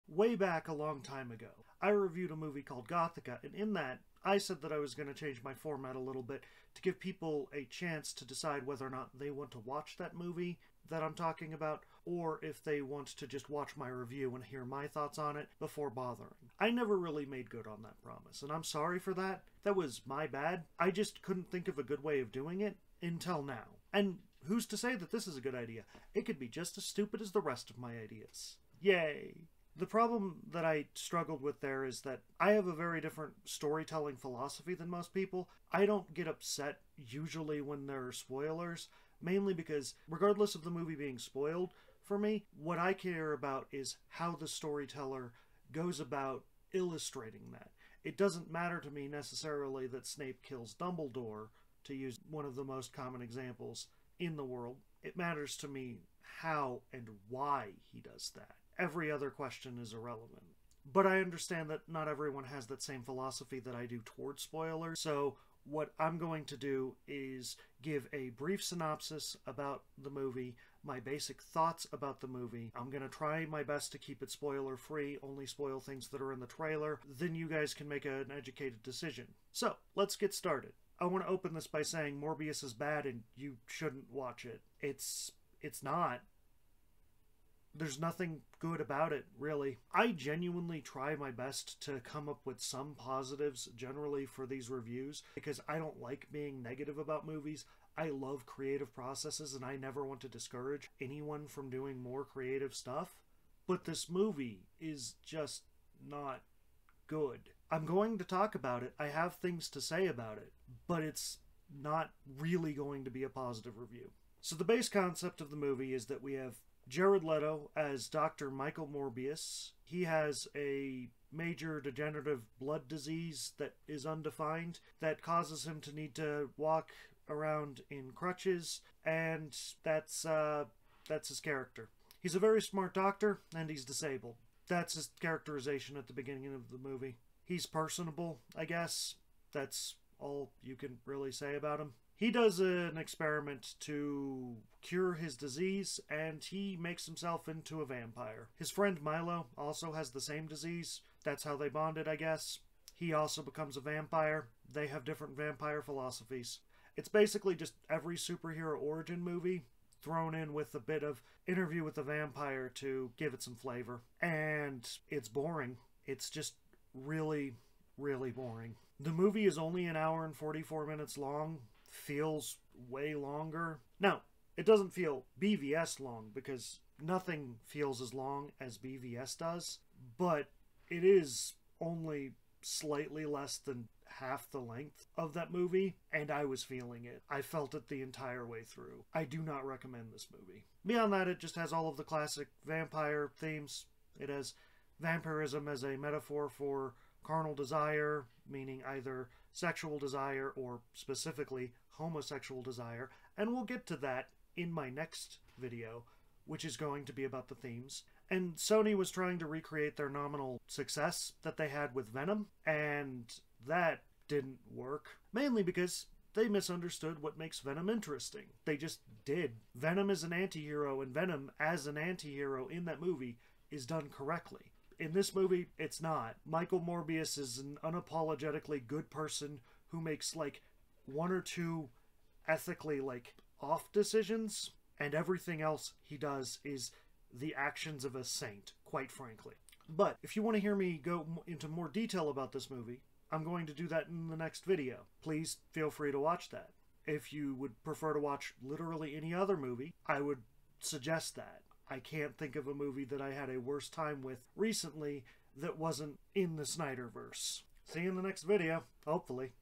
Morbius? More Like More Boring Us! | Why "Morbius" (2022)? | Spoiler Free Review
I know that joke in the title is bad, but it's still funnier than anything in the actual movie. Full review is coming. I'm just further developing my style and this might help with the fact that I don't do weekly uploads.